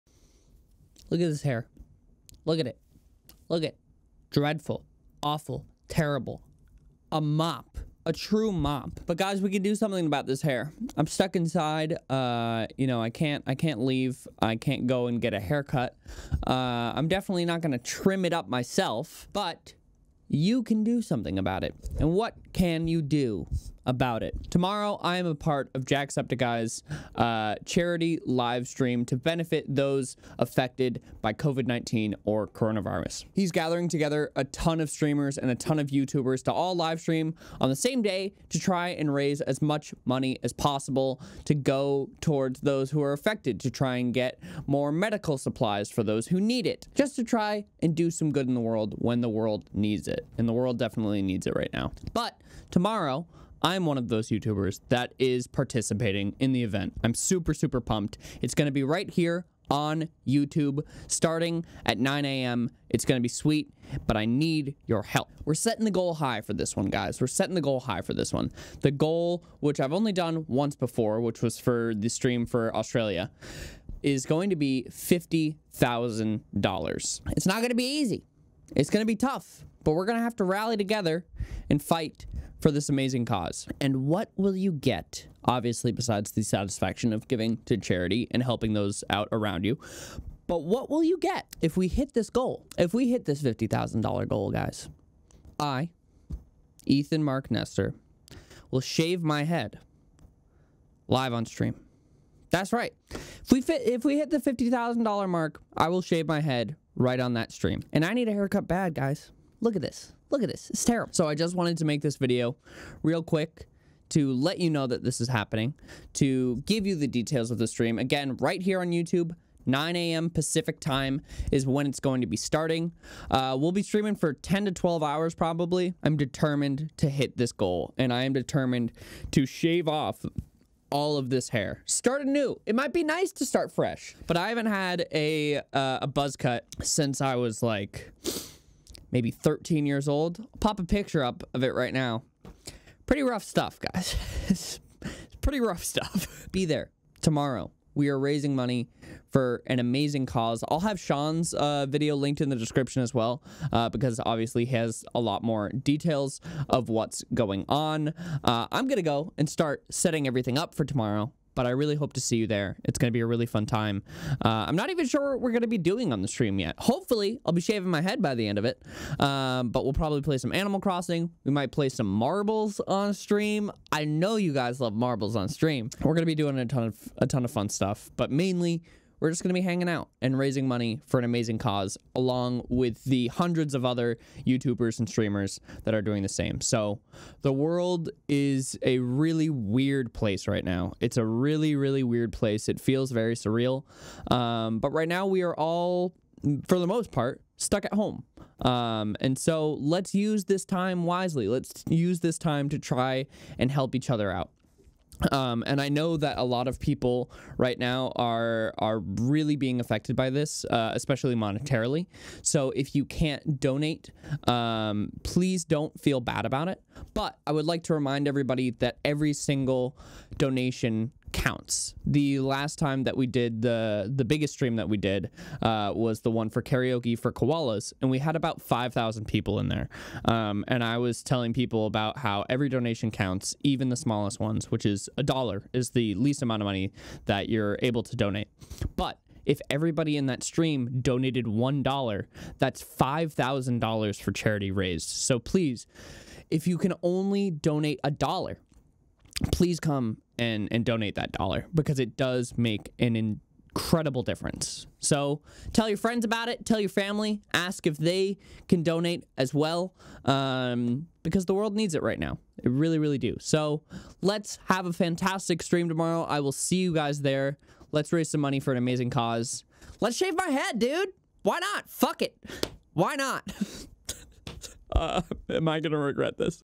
I'm Shaving My Head for Charity
With your help, I will shave my head for charity
Stream starts tomorrow, April 7th at 9am PST here on YouTube!